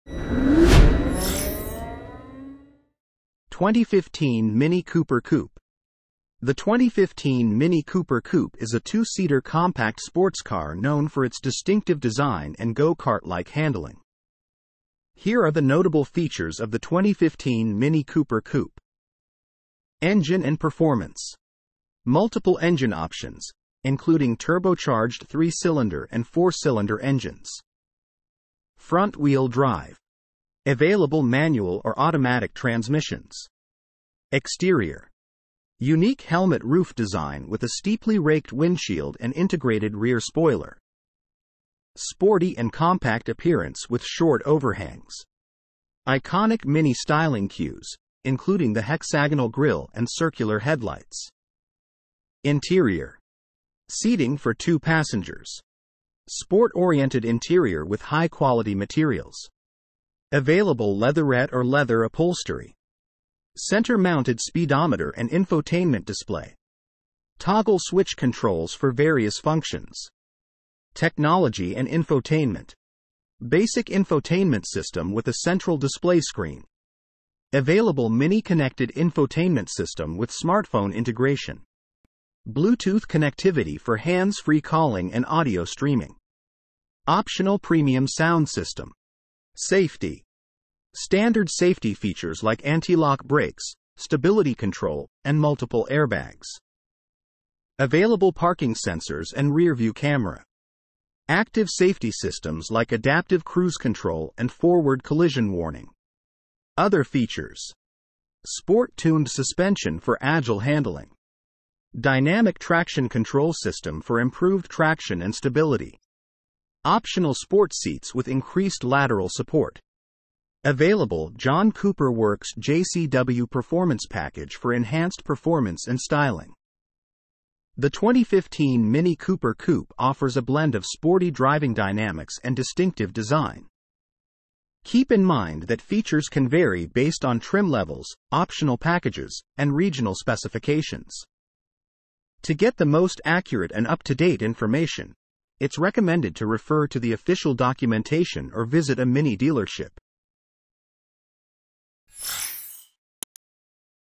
2015 Mini Cooper Coupe
The 2015 Mini Cooper Coupe is a two-seater compact sports car known for its distinctive design and go-kart-like handling.